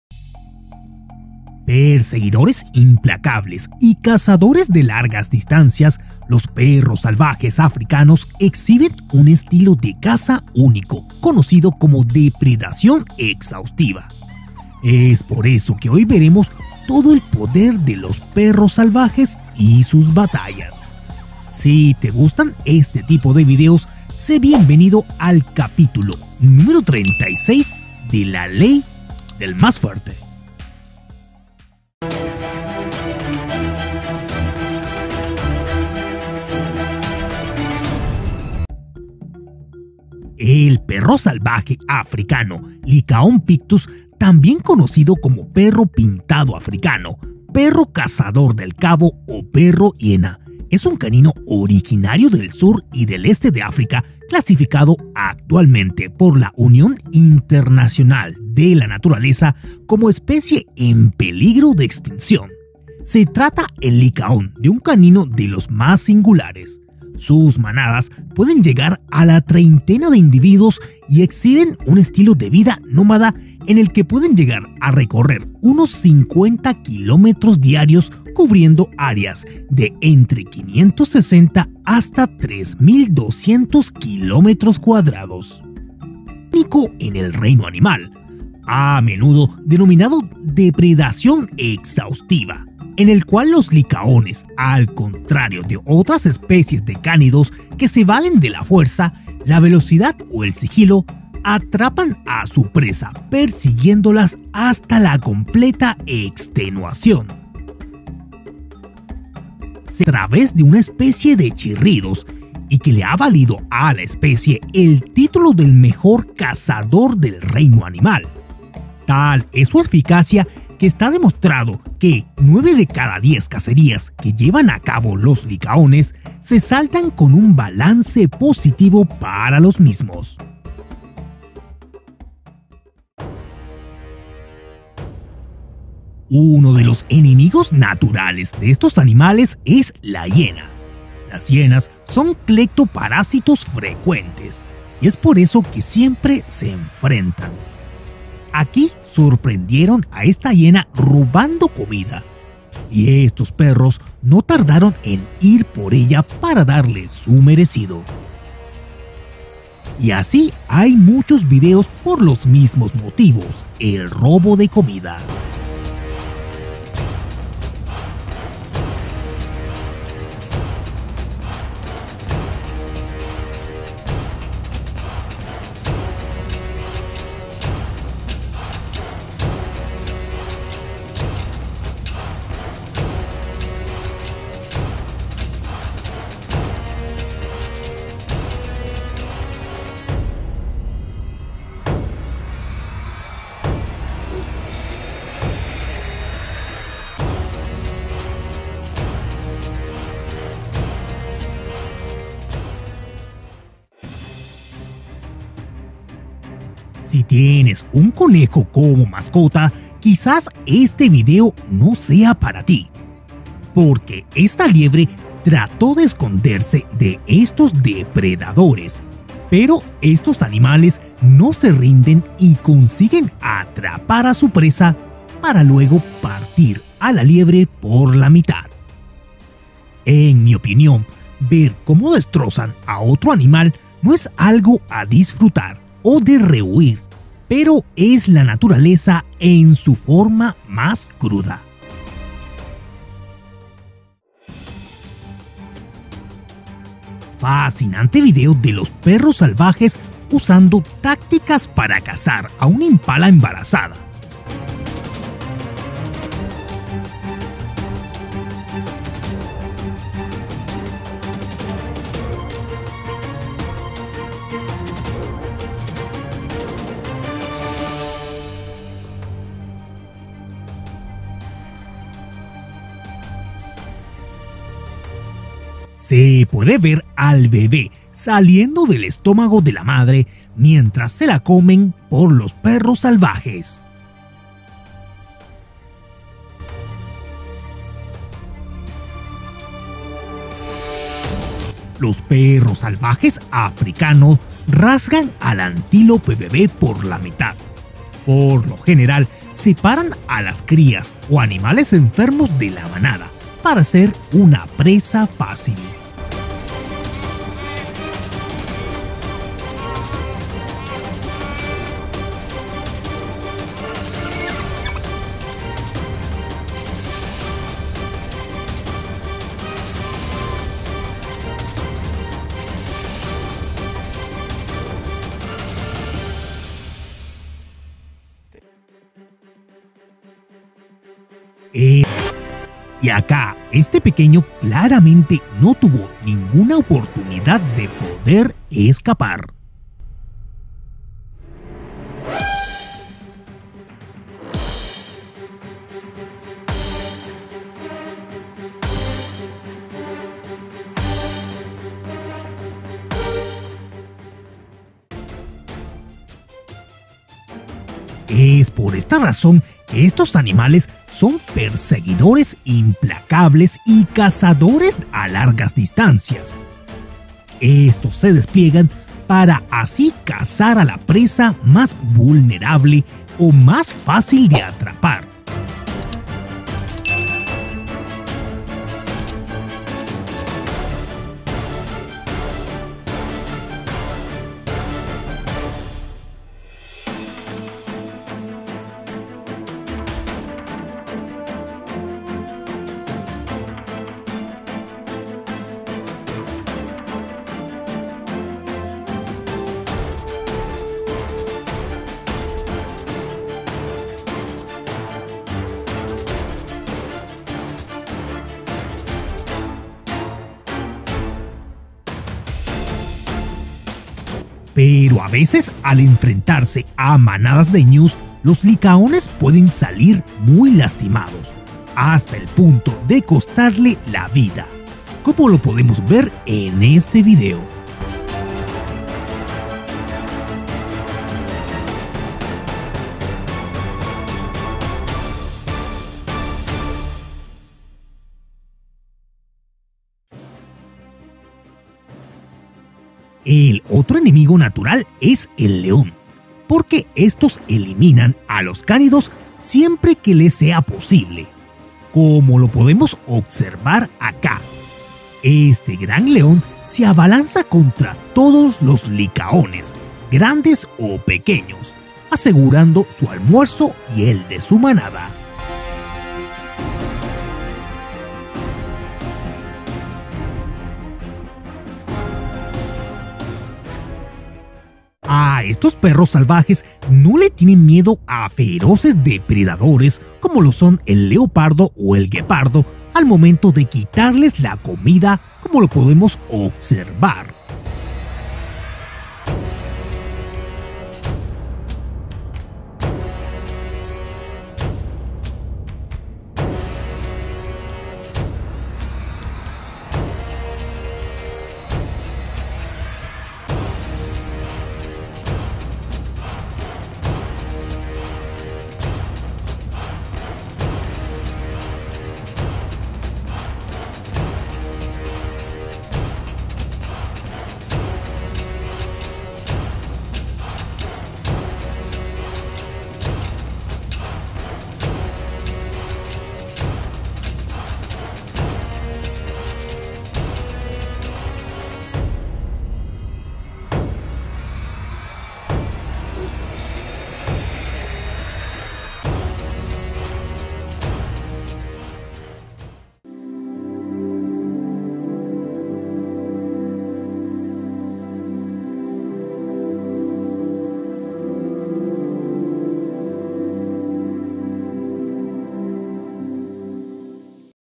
EL PODER DE LOS PERROS SALVAJES, El mejor cazador del reino animal
Perseguidores implacables y cazadores de largas distancias, los perros salvajes africanos exhiben un estilo de caza único conocido como depredación exhaustiva,  ES por eso que hoy veremos todo el poder de los perros salvajes  y sus batallas, si te gustan este tipo de videos se bienvenido al capitulo numero 36 de la ley del mas fuerte.

El perro salvaje africano -Lycaon pictus- también conocido como perro pintado africano, perro cazador del Cabo o perro hiena, es un cánido originario del sur y este de África clasificado actualmente por la Unión Internacional de la Naturaleza  como especie en peligro de extinción. Se trata el licaón de un cánido de lo más singular: sus manadas pueden llegar a la treintena de individuos y exhibe un estilo de vida nómada en el que puede llegar a recorrer unos 50 kilómetros diarios cubriendo áreas de entre 560 hasta 3200 kilómetros cuadrados.

Sin embargo, los perros del Cabo son conocidos por su coordinado y sofisticado estilo de caza, único en el reino animal, a menudo denominado depredación exhaustiva, y en el cual los licaones —al contrario de otras especies de cánidos que se valen de la fuerza, la velocidad o el sigilo— atrapan a sus presas persiguiéndolas hasta la completa extenuación.

Se trata de hecho de un método de caza tremendamente efectivo, en el que algunas manadas deciden como cazar comunicando su voto a través de una especie "chirridos", y que le ha valido a la especie el título del mejor cazador del reino animal. Tal es su eficacia que está demostrado que 9 de cada 10 cacerías que llevan a cabo los licaones se salda con un balance positivo para los mismos.

@TOP MAS   @LEY DEL MÁS FUERTE

Videos sugeridos

EL PODER DEL DRAGON DE KOMODO VS bufalo, caballo, ciervo, mono, cobra | LEY DEL MÁS FUERTE